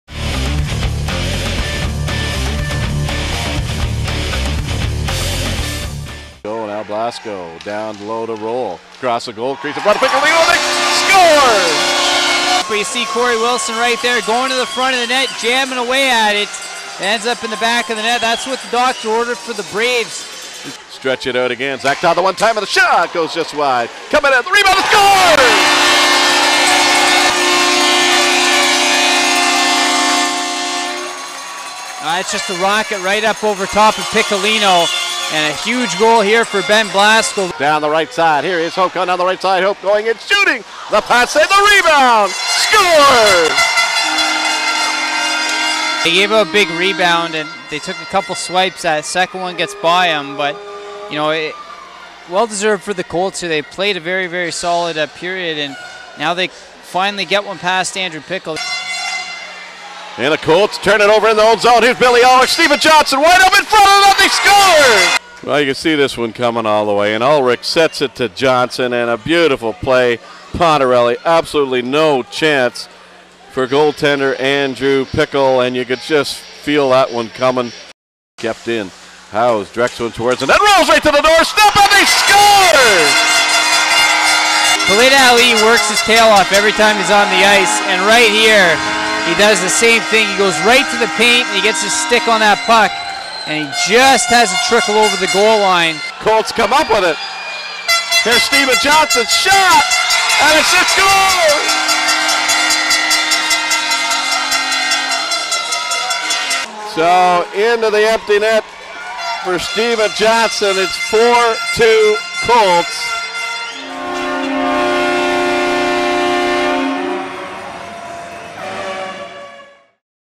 CCHL Highlights Mar 22 2013
Central Canada Hockey League Highlights from Friday March 22 2013. Game 6 of the first round between the Cornwall Colts & the Brockville Braves from the Brockville Memorial Centre.